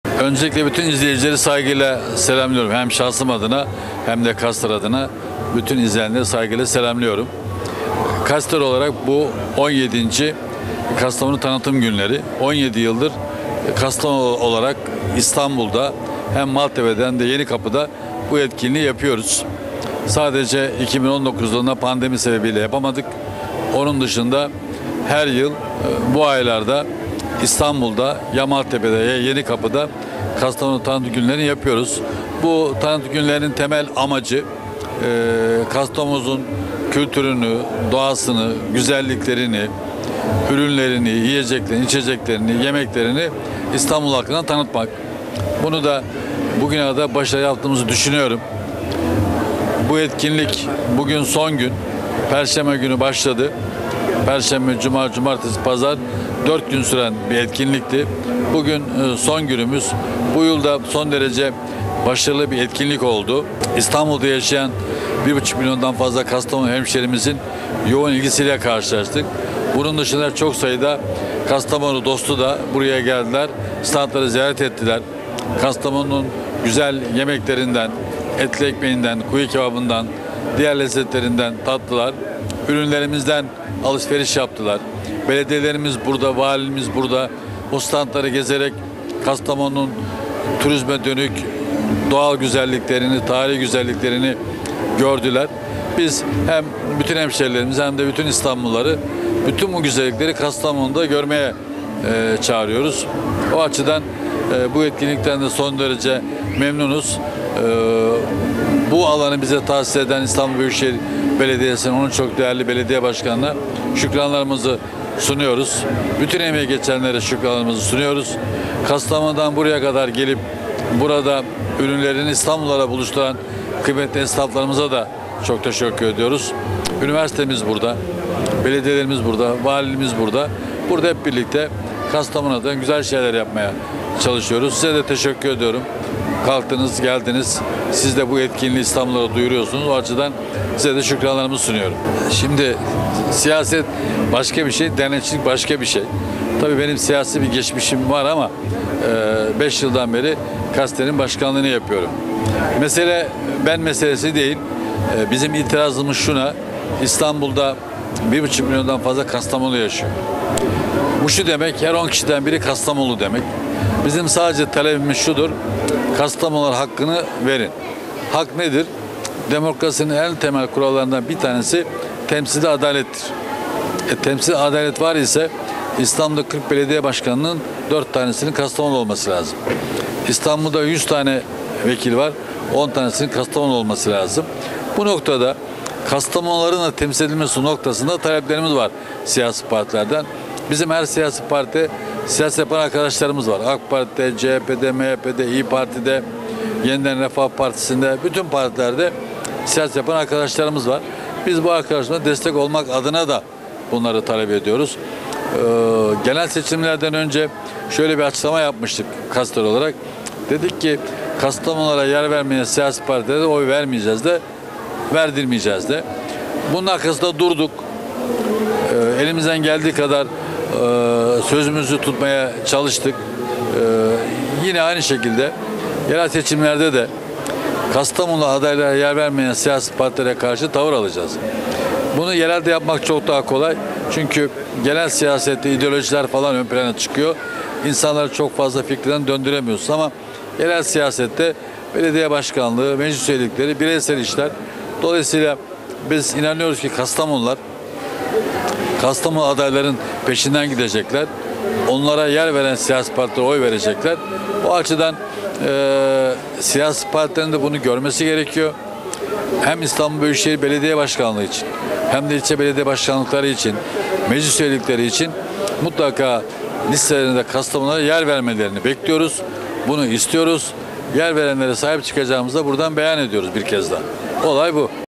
Kastamonu Günleri Başkan Remzi Şen
KAS -DER Genel Başkanı Remzi Şen 17. Kastamonu Günleri hakkında açıklama yapıyor